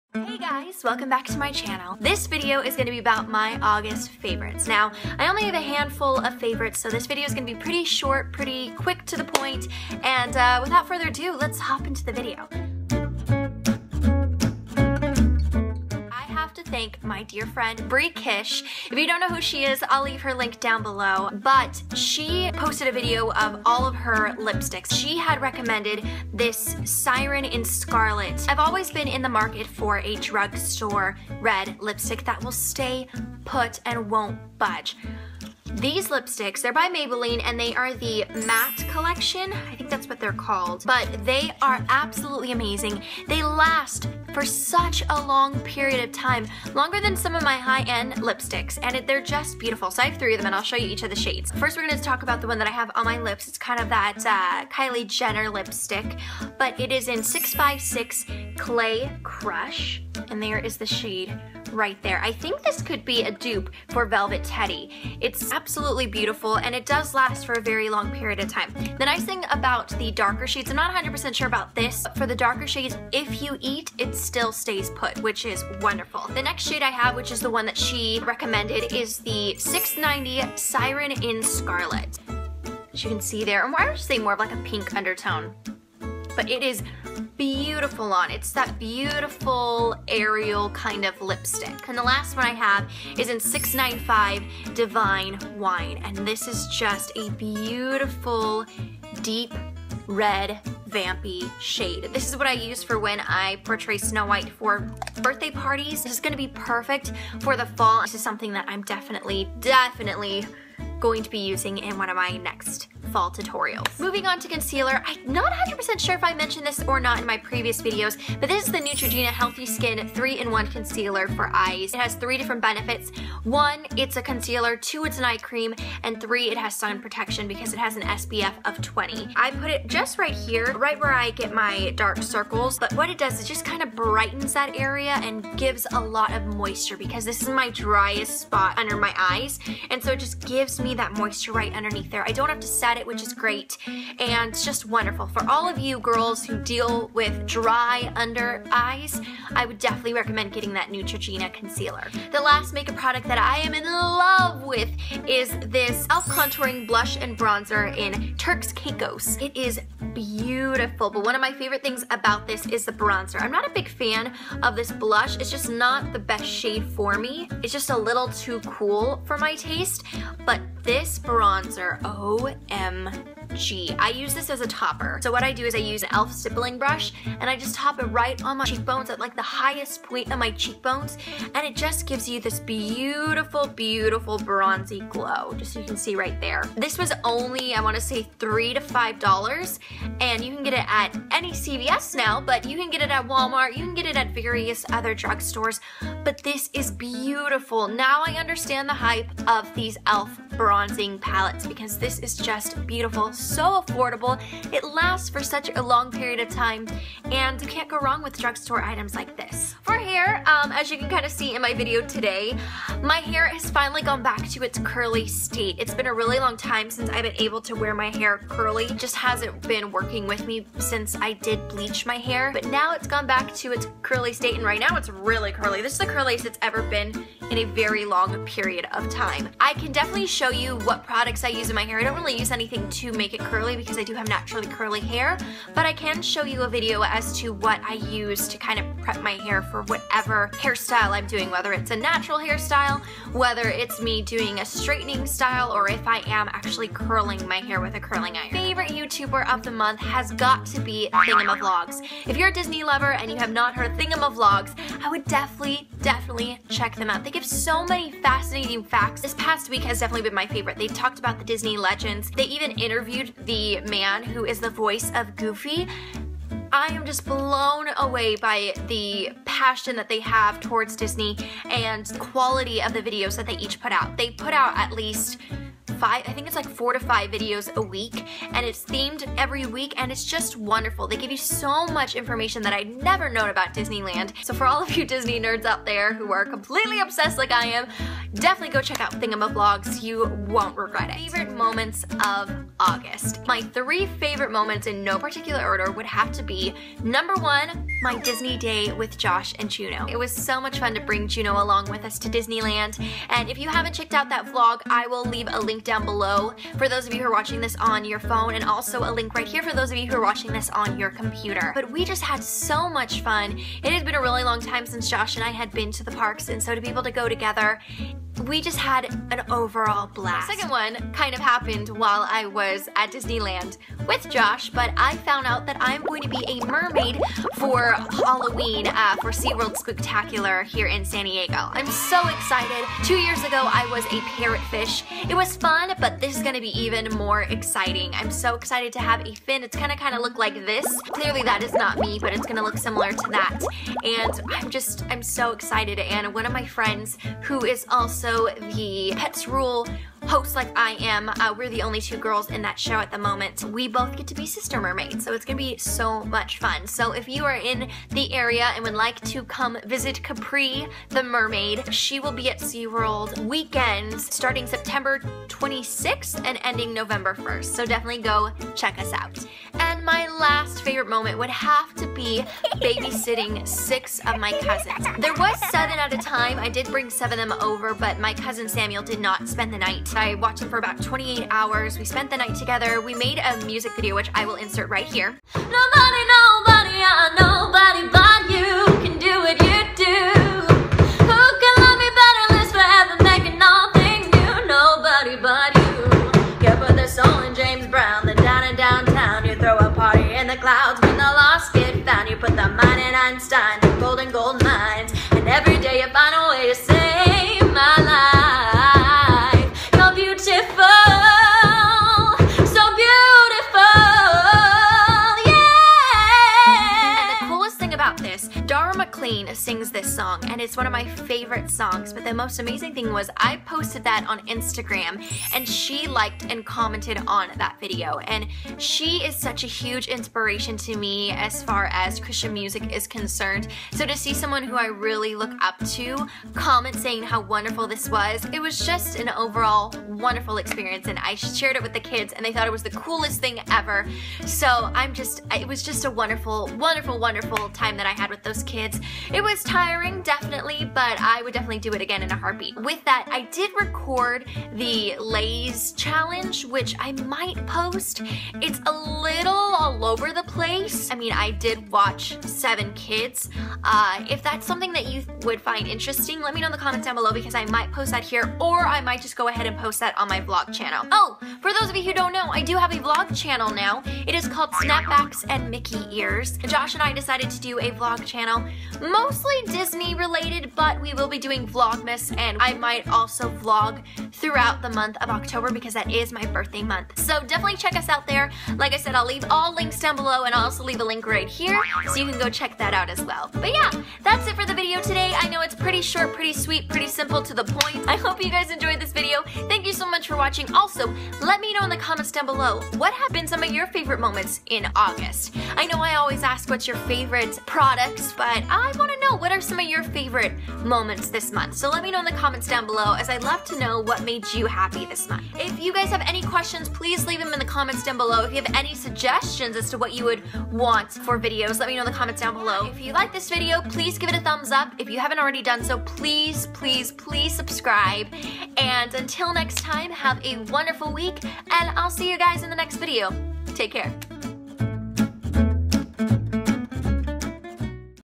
August Favorites 2015 (Drugstore Makeup, Thingamavlogs, Sea World, + More!)
I can't believe we are in September!! What have been some of your favorite moments in August?

Favorites Mentioned In Video:
Maybelline Color Sensational Creamy Matte Lipsticks (Clay Crush, Siren in Scarlett, Divine Wine)
Neutrogena 3-IN-1 Concealer For Eyes- Fair
E.L.F. Contouring Blush + Bronzer- Turks Caicos
Thingamavlogs
Curly Hair
3 Favorite Moments